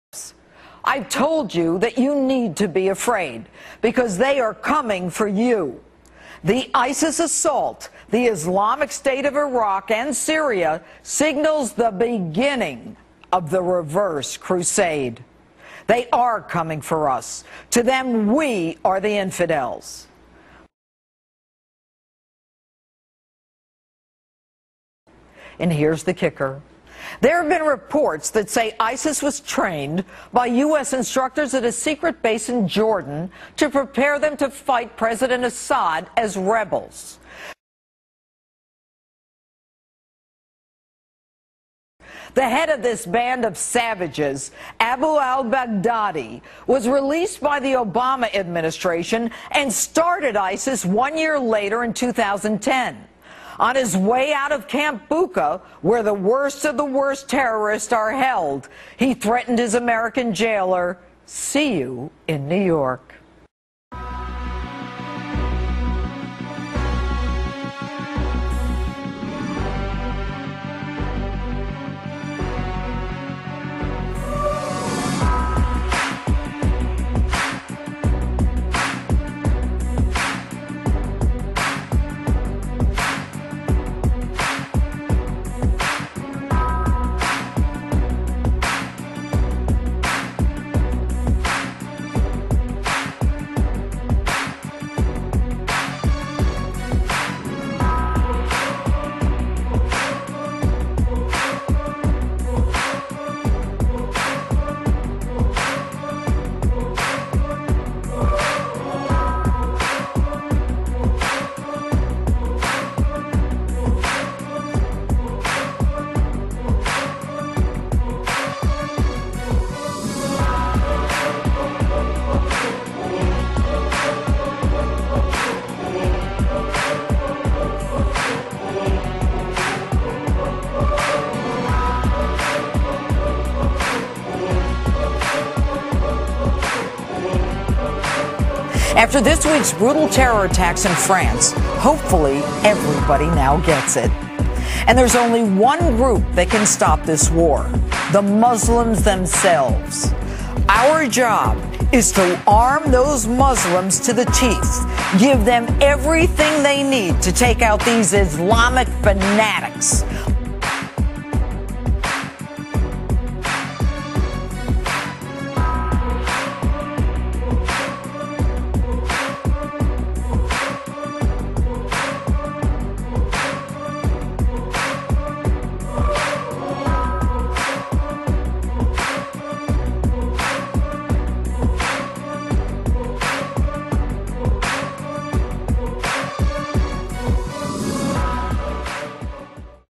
I'm a Canadian Muslim and I Hate War. Use Quran to Defeat ISIS
Canada should bring our troops back. Let the tyrants who made ISIS fix their "unintentional" mistakes.